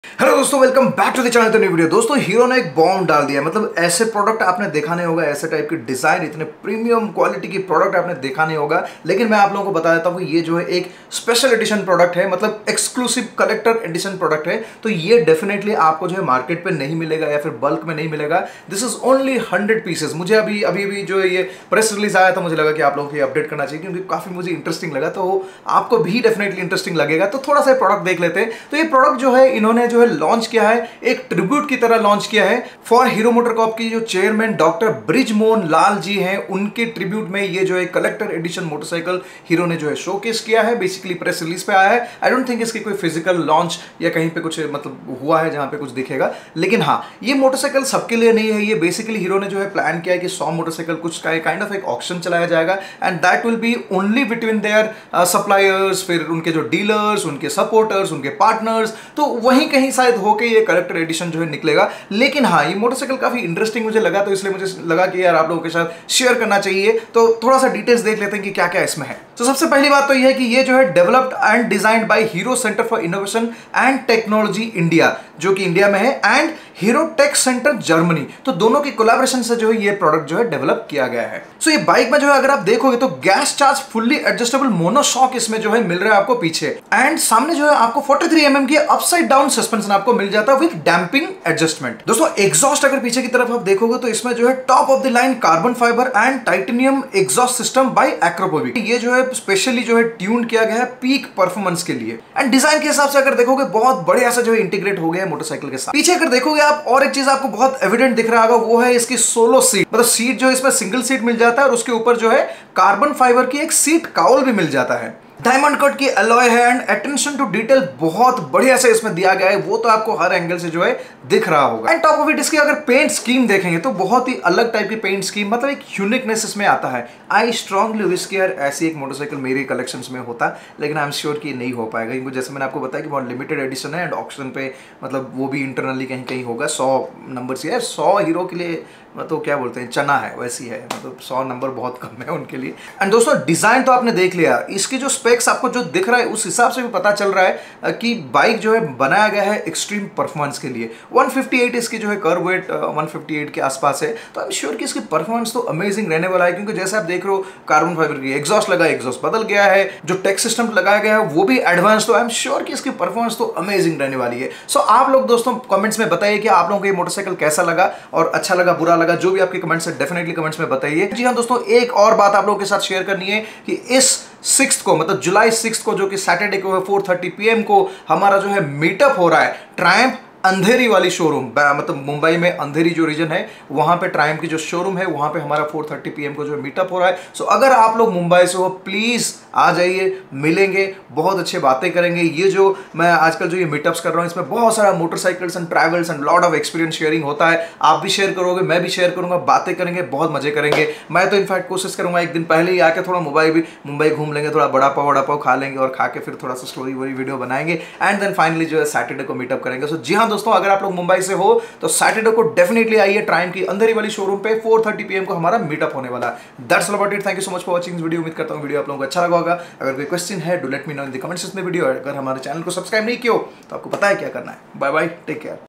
HERO ne BOMB Daal Diya - Limited Edition Motorcycle
The Limited Edition Motorcycle from Hero Motorcorp - The Centennial.
Mumbai Meetup - Triumph, Andheri on 6th July at 4.30 PM